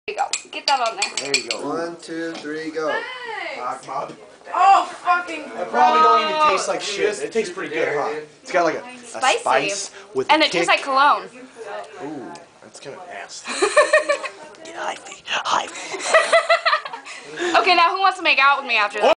How burl are you? Sierra again....
Here is Sierra drinking a mix of Redline, Monster, Cologne & Tabasco for some spice!